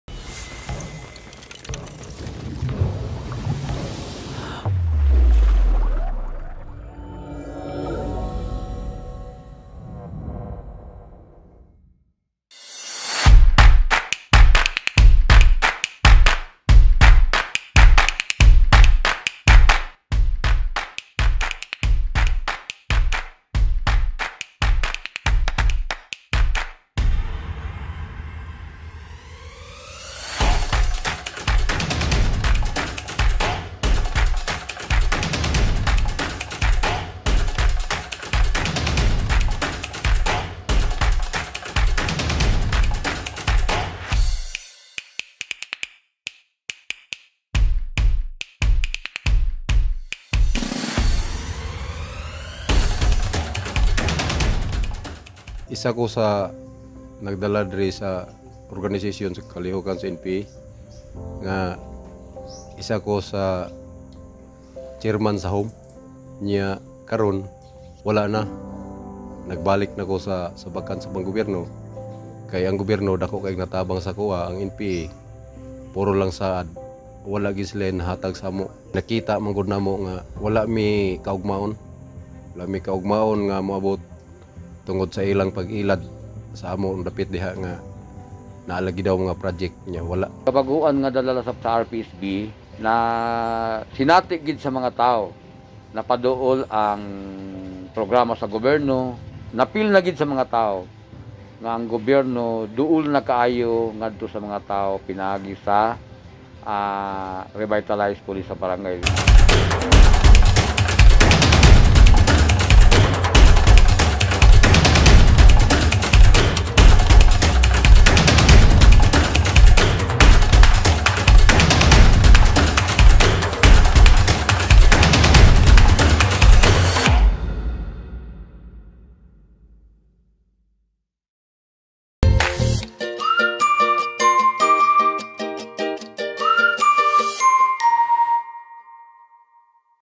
Revitalized Pulis Sa Barangay: Highlights of Community Impact
"Join us as we embark on a journey through the impactful strides of Revitalized Pulis Sa Barangay (R-PSB). Explore the heartwarming stories and tangible changes witnessed in communities touched by R-PSB initiatives. From fostering safer neighborhoods to empowering local youth and families, R-PSB has become synonymous with positive transformation. Through collaborative efforts and unwavering dedication, R-PSB continues to redefine the landscape of community policing, leaving a lasting legacy of hope, trust, and resilience in its wake. Step into the world of R-PSB and witness the profound difference it makes, one barangay at a time."